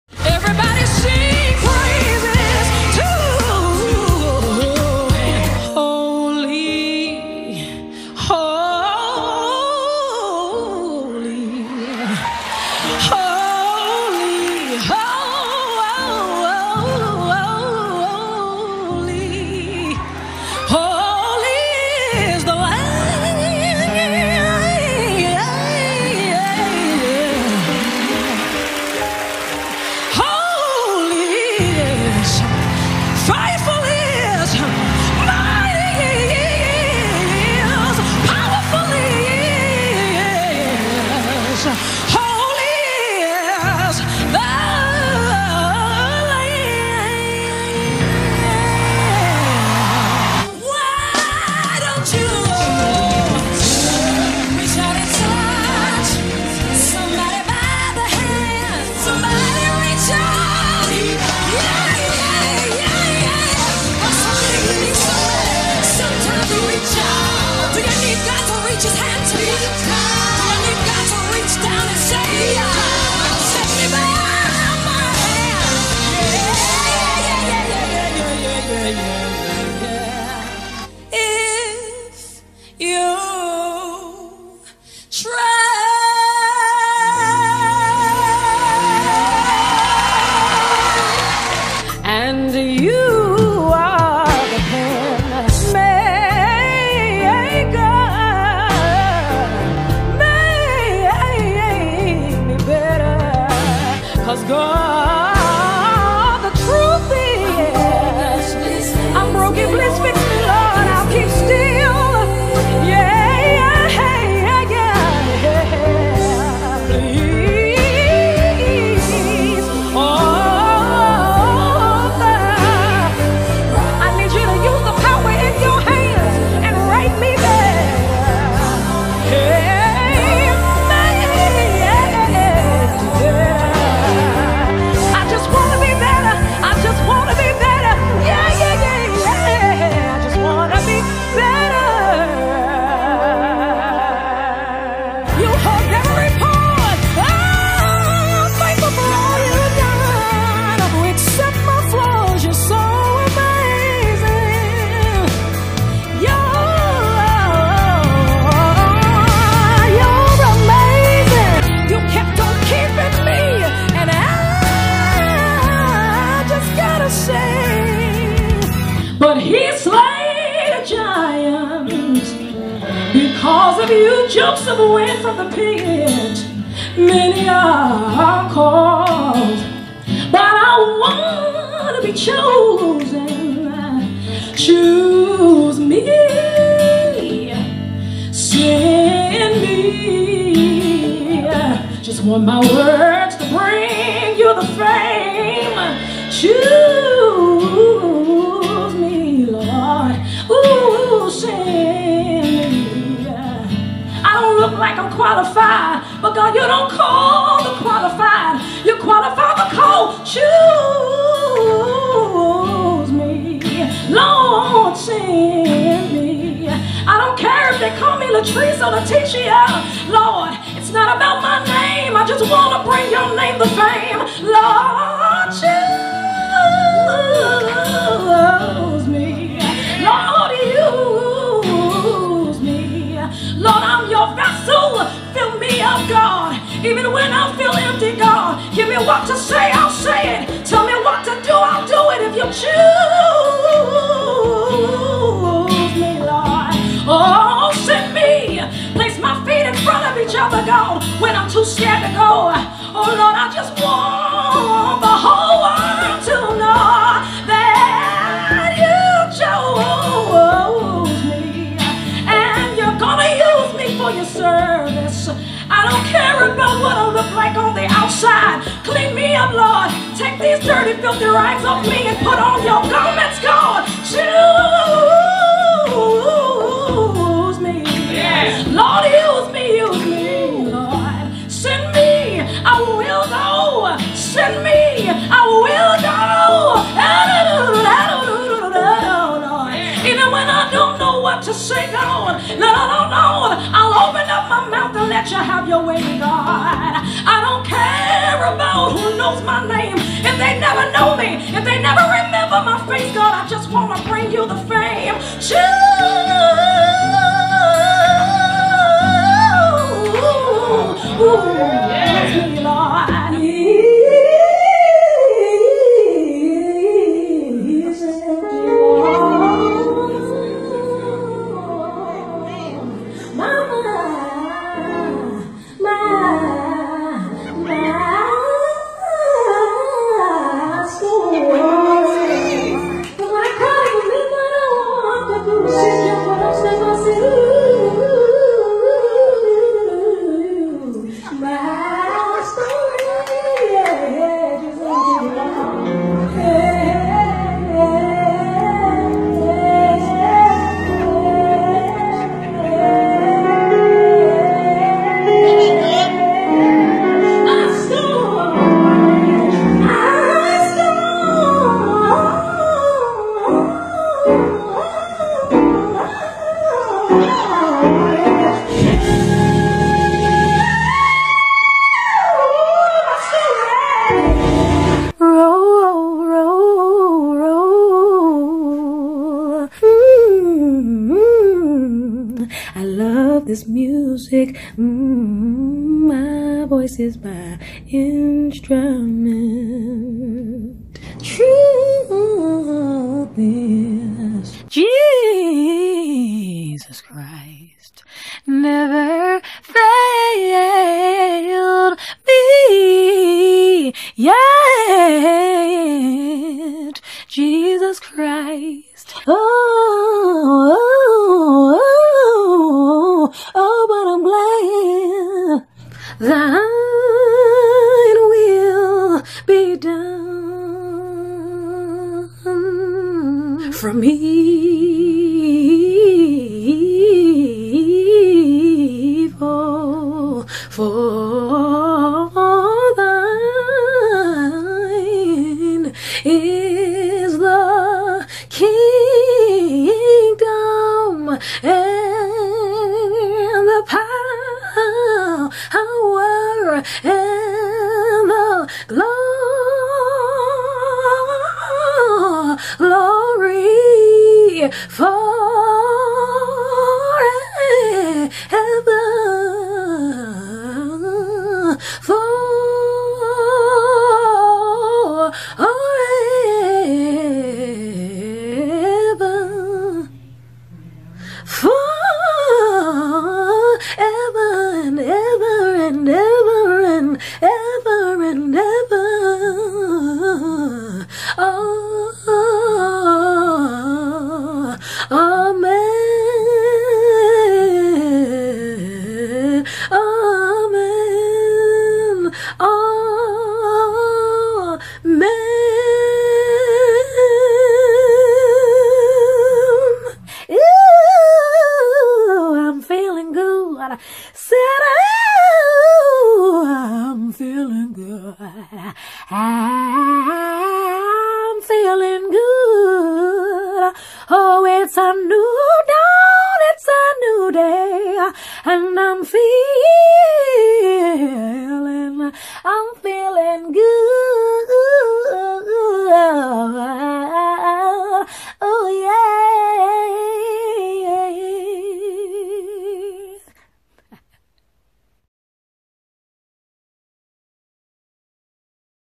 (Latice Crawfor - Best Vocals)QUANDO JUNTA TÉCNICA VOCAL COM UNÇÃO
@Latice Crawford  - Compilação de melismas,drives, agudos

Negra, Black Vocal,gospel,soul,cantando com a alma,best vocals